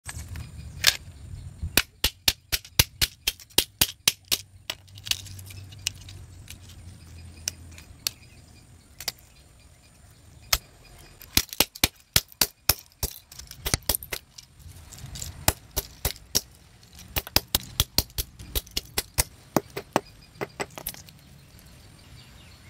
Found an old joystick 🫢🎮🔨
Here we are crushing an old video game joystick to see what is in it.